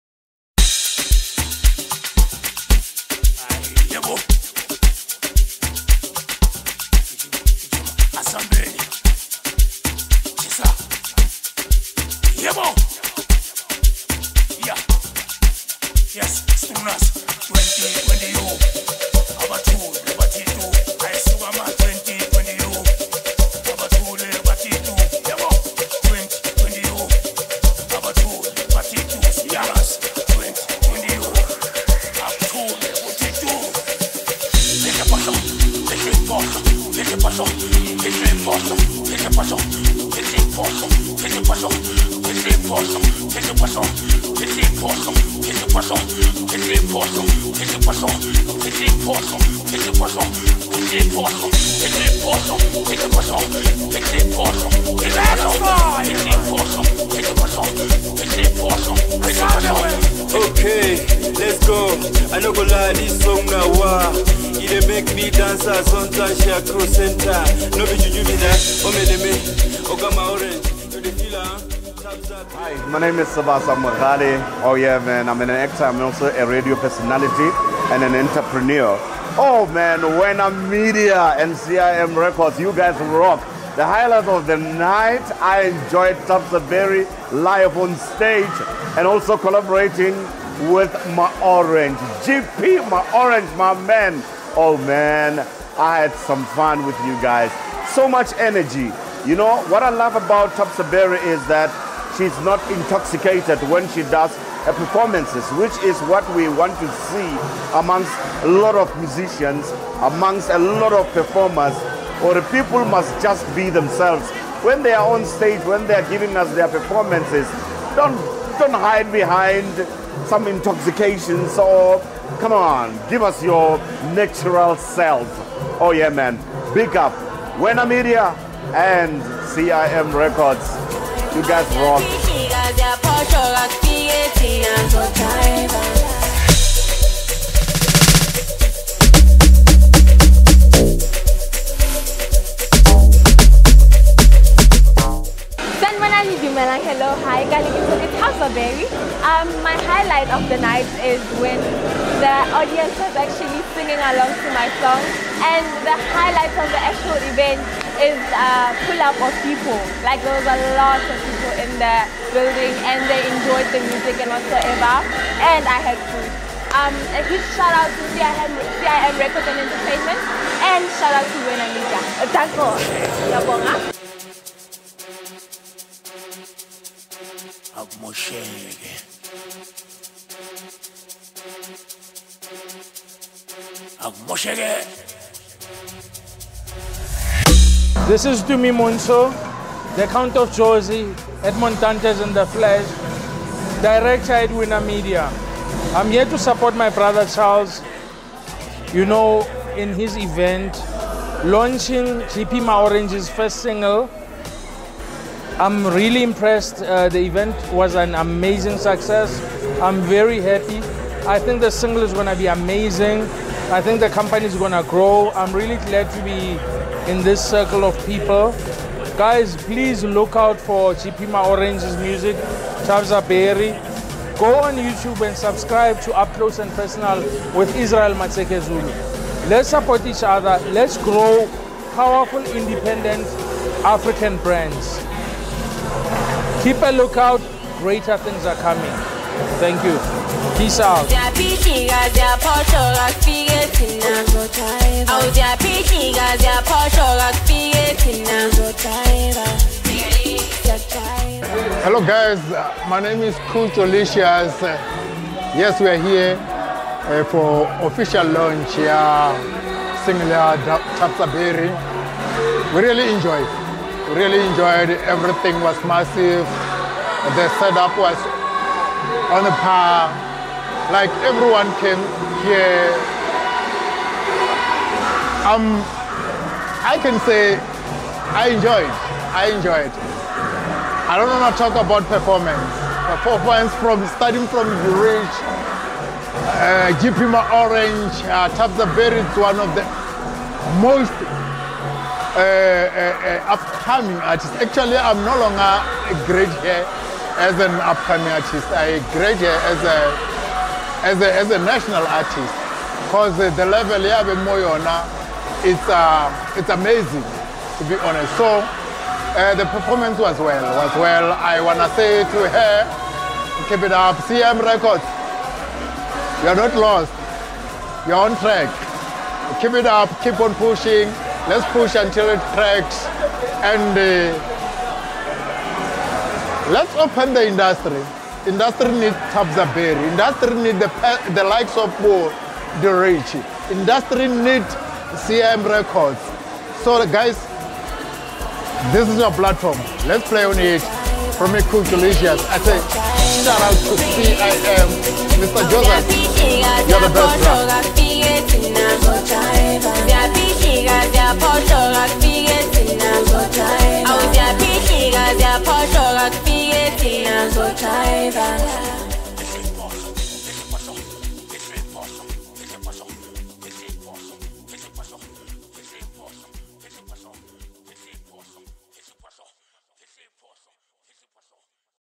Israel Matseke Zulu, GP MaOrange, Phihli Phohlo, Thabza Berry
GP MaOrange Official Single Launch at Imbizo Shisanyama Mall of Tembisa
Ft Thabza Berry × Du Richy & Distructive Twins
Shot by Media Tour Films
Directed by Reuben Mokolo
CIM Records and Entertainment
Special Guests:
Sebasa Mogale
Itumeleng 'Etubeats' Montsho
Khutso 'Cutjolicious' Chuene
Mzwakhe Sigudhla
Sfiso 'Fistos' Sigudhla
etc